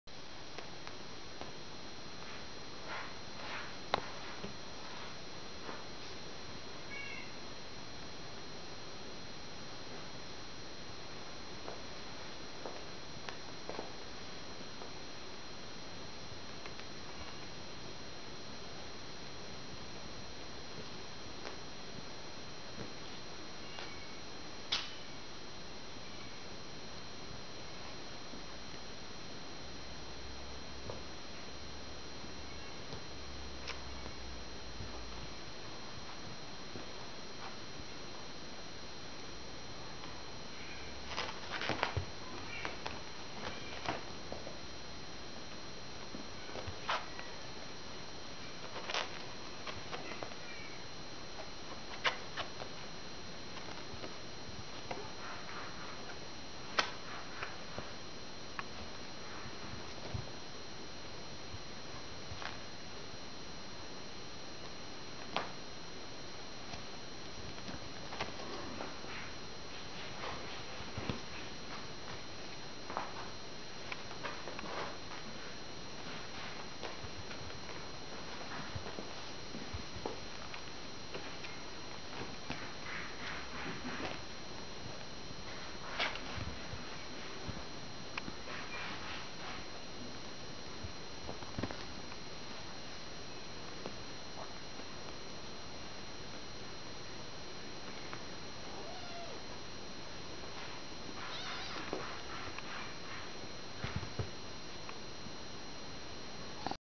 Micini Miette, seconda parte
Qui Yamirez è bello vispo, attacca il topino di pezza mentre Yari e Yalil sono impegnati nella lotta corpo a corpo!